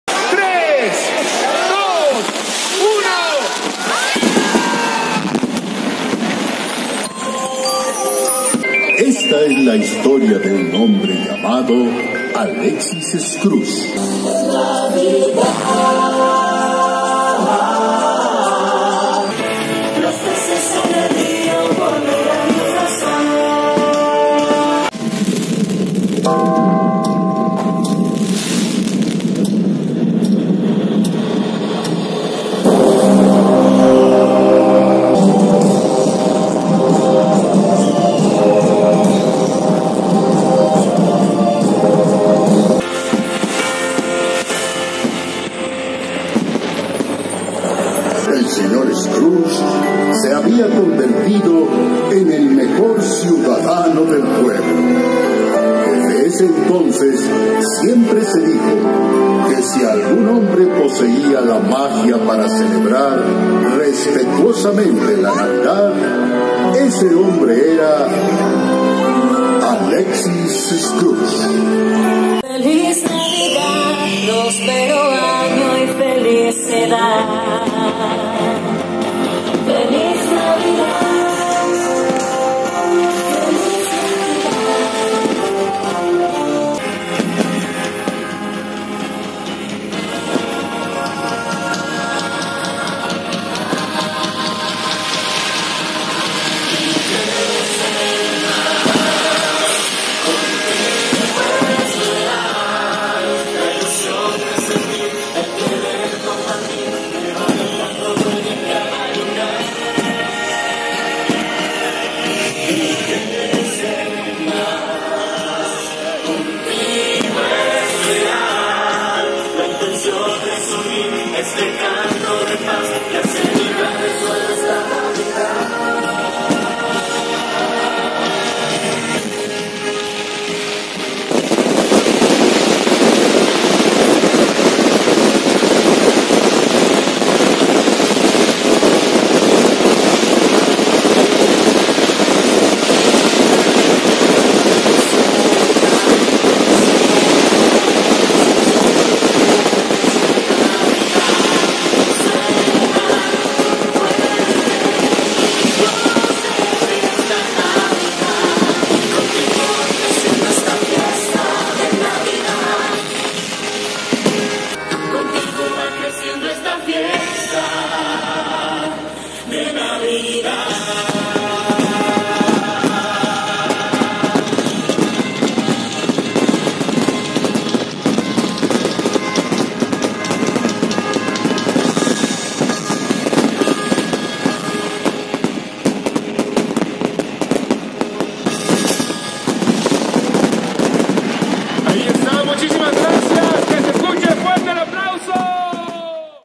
Espectacular pirotecnia 🎇 en el encendido de Árbol de Navidad en Pachuca 2018
Con música, magia y mucha pirotecnia; así se vivió el tradicional encendido de Árbol en Pachuca.